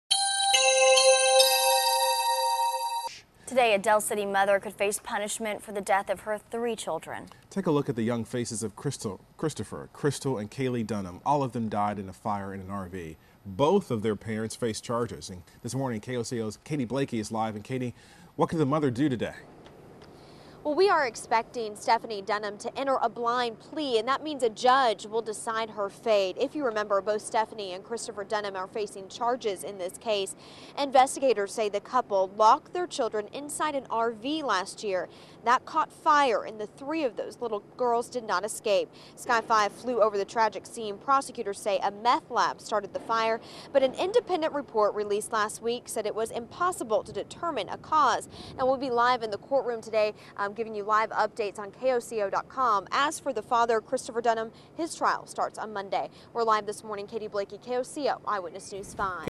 Stephanie Dunham Headed To Court
Stephanie Dunham could enter a blind plea in court today. She's charged in connection with the death of her children.